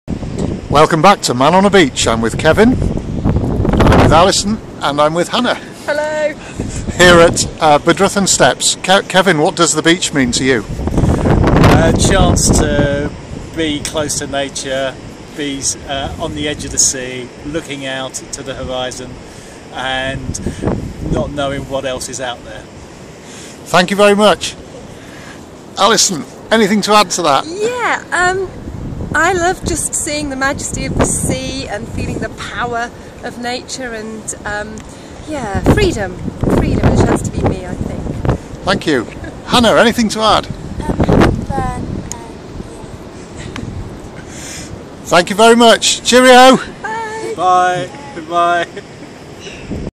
Bedruthan Interview 2
With Kevin and Alison, at Bedruthan Steps in North Cornwall, from manonabeach.